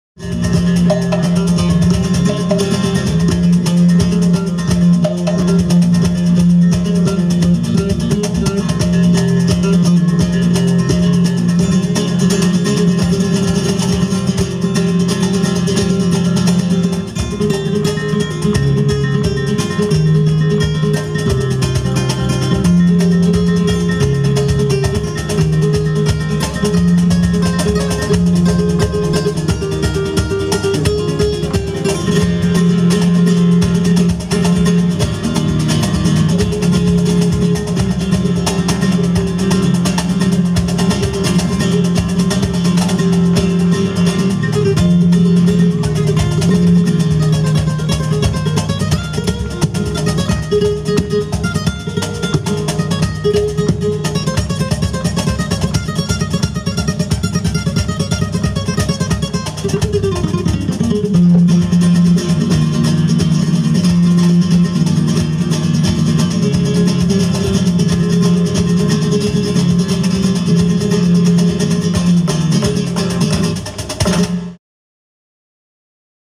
SPOT SEMIFINAL CONCURSO PETENERAS 2018. videolozano.es
8 SEMIFINALISTAS - SÁBADO 21 DE JULIO A LAS 22:00H EN LA PLAZA DE LA CONSTITUCIÓN DE  PATERNA DE RIVERA (CUNA DE LA PETENERA). videolozano.es